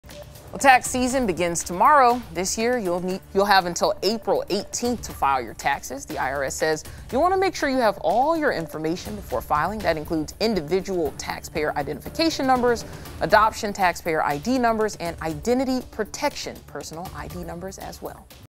Tax season begins Monday | What you need to know
As the new year rolls in, so does tax season.

The Internal Revenue Service will begin accepting and processing tax returns on Jan. 23, kicking off the 2023 tax season. Americans will have until April 18 to file their tax returns, granting them an extra three days from the usual deadline.

The extra days happened because the normal filing date of April 15 falls on a weekend and the District of Columbia’s Emancipation Day holiday falls on April 17.